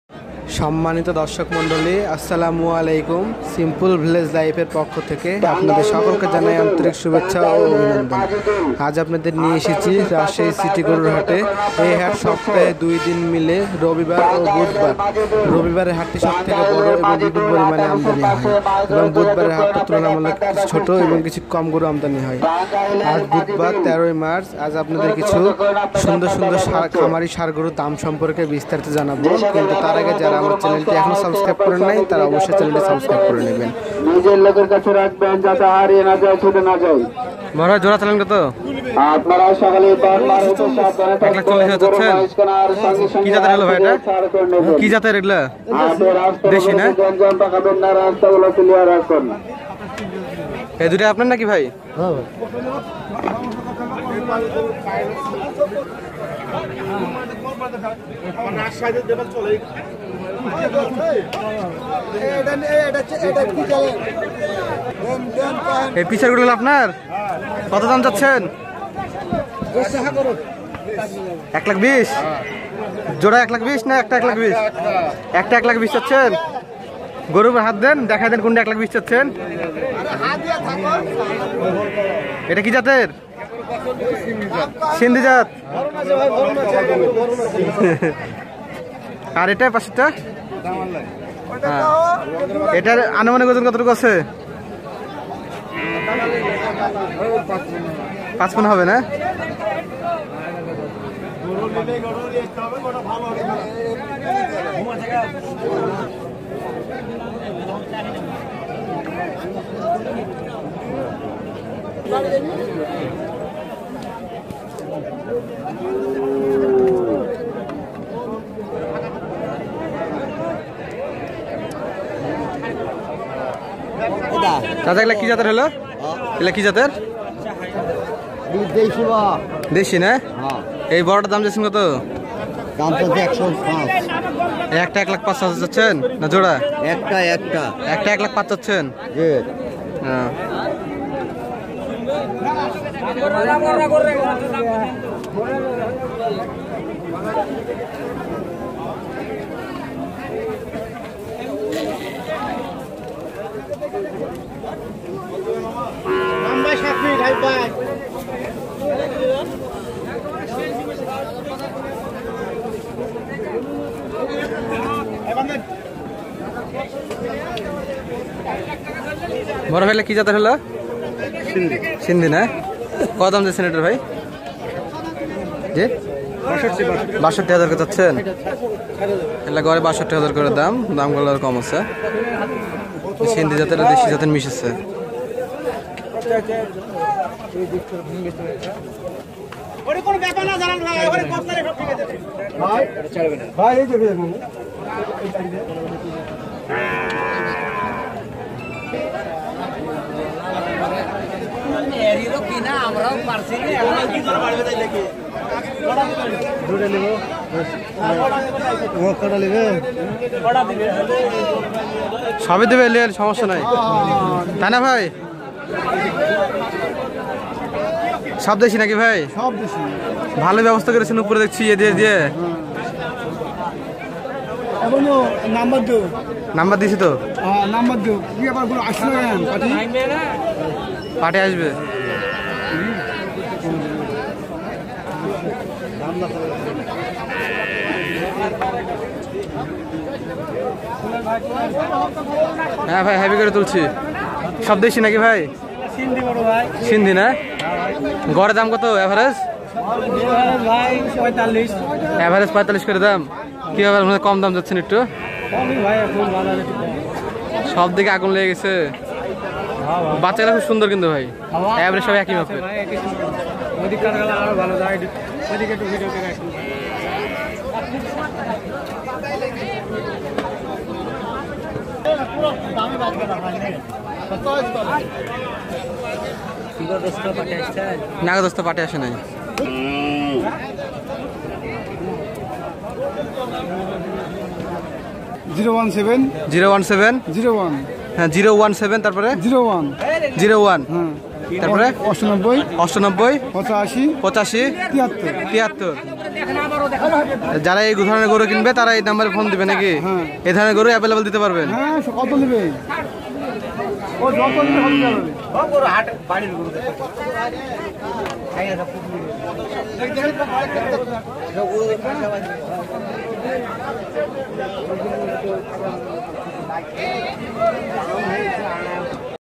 796| Cow-Calves market price|সর্বশ্রেষ্ঠ খামারি ষাঁড় গরুর মূল্যসহ বিক্রেতার সাক্ষাৎকার দেখুন
Cow-Calves market price|সর্বশ্রেষ্ঠ খামারি ষাঁড় গরুর মূল্যসহ বিক্রেতার সাক্ষাৎকার দেখুন